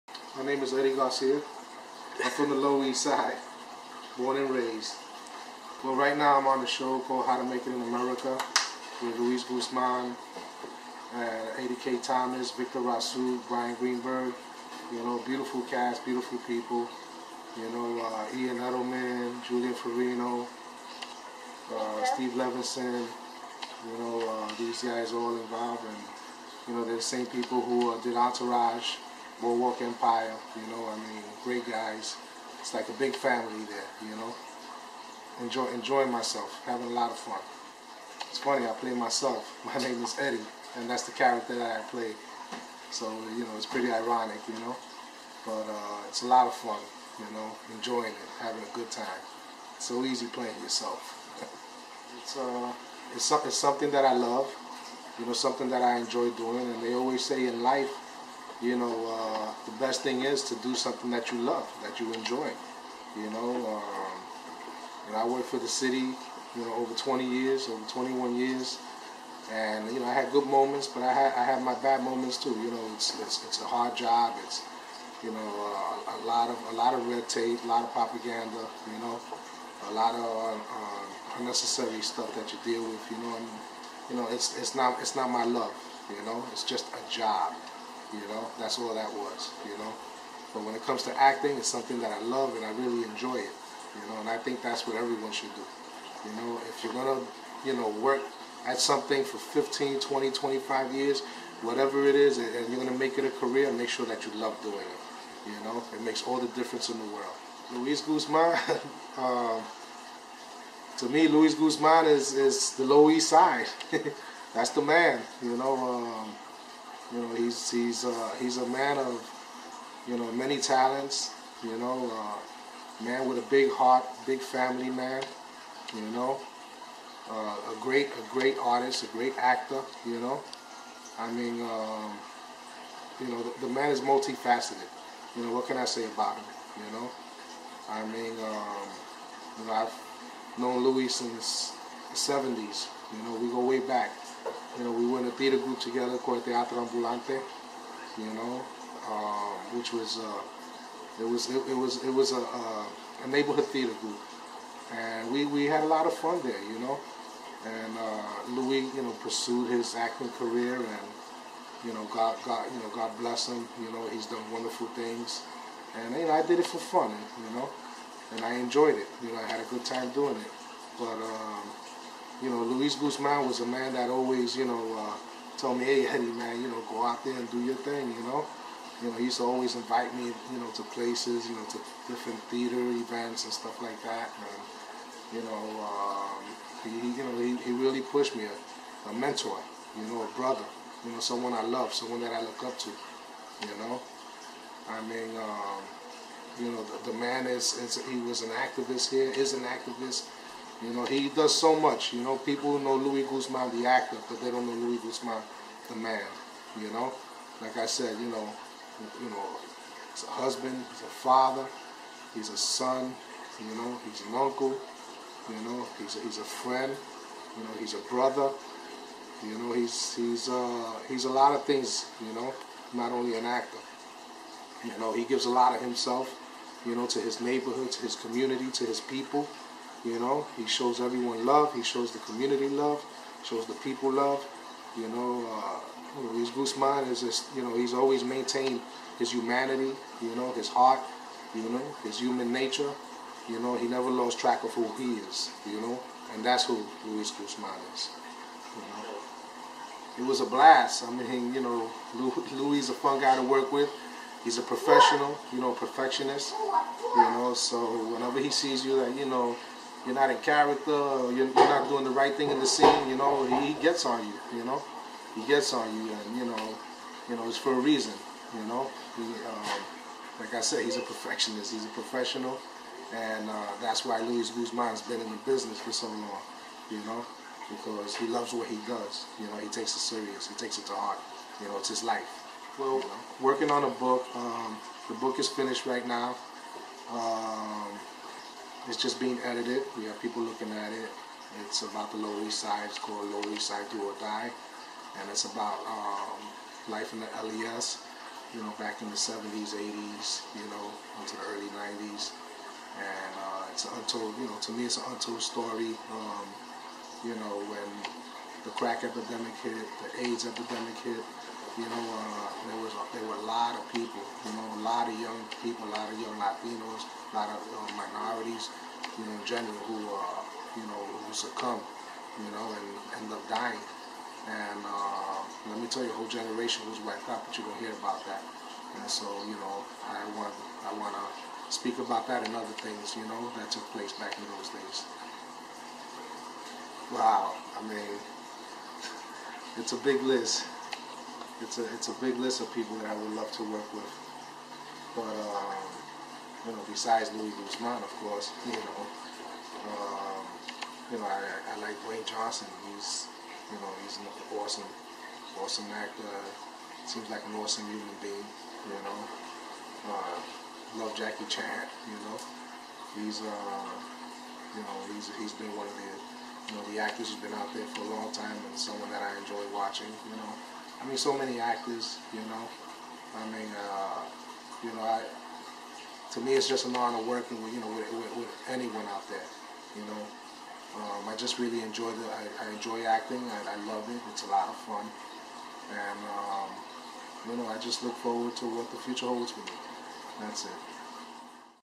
Interview with Actor Edgar Garcia
LuisGuzmanTV interviews Actor Edgar Garcia currently featured on How To Make It In America.